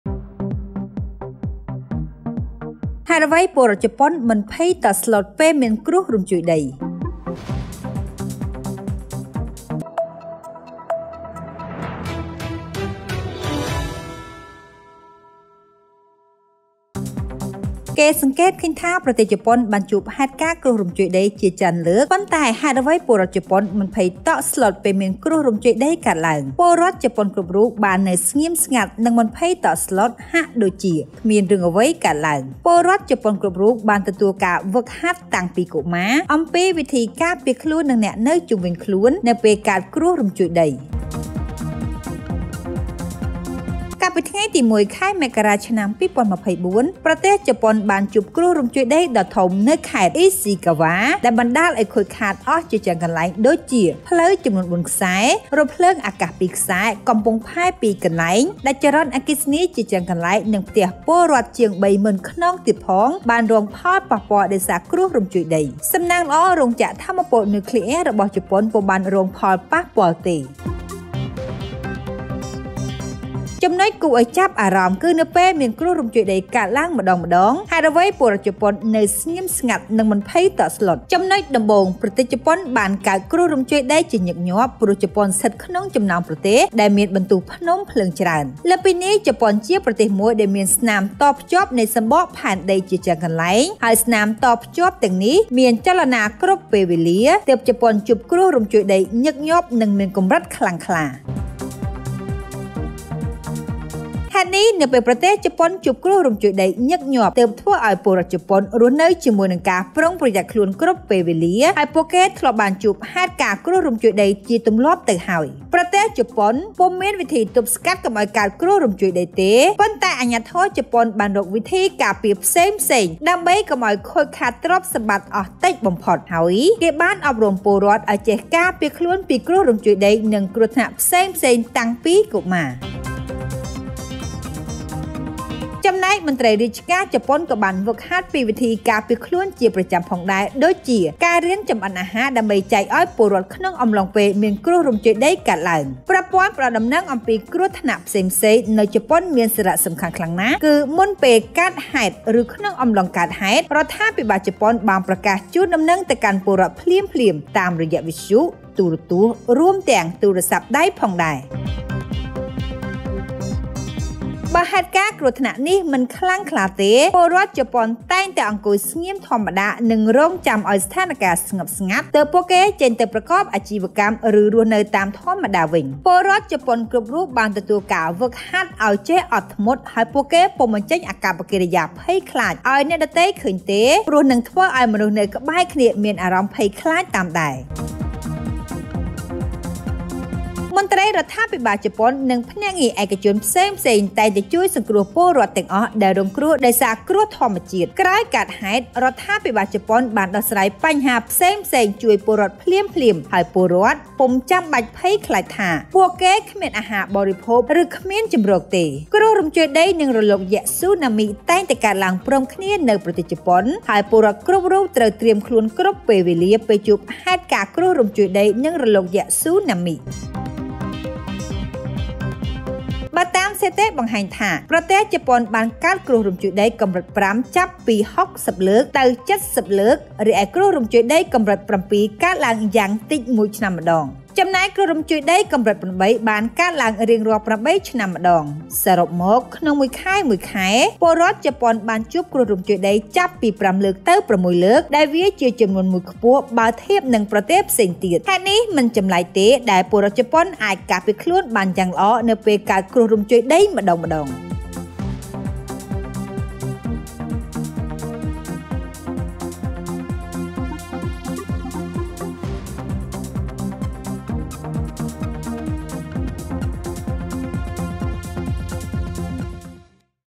ហេតុអ្វីពលរដ្ឋជប៉ុនមិនភ័យតក់ស្លុតពេលមានគ្រោះរញ្ជួយដី?
ប្រទេសជប៉ុន ៖ គេសង្កេតឃើញថា ប្រទេសជប៉ុនបានជួបហេតុការណ៍គ្រោះរញ្ជួយដីជាច្រើនលើក ប៉ុន្តែហេតុអ្វីពលរដ្ឋជប៉ុនមិនភ័យតក់ស្លុតពេលមានគ្រោះរញ្ជួយដីកើតឡើង? ពលរដ្ឋជប៉ុនគ្រប់រូបបាននៅស្ងៀមស្ងាត់ និងមិនភ័យតក់ស្លុត ហាក់ដូចជាគ្មានរឿងអ្វីកើតឡើង ។ ពលរដ្ឋជប៉ុនគ្រប់រូបបានទទួលការហ្វឹកហាត់តាំងពីកុមារ អំពីវិធីការពារខ្លួន និងអ្នកនៅជុំវិញខ្លួន នៅពេលកើតគ្រោះរញ្ជួយដី ។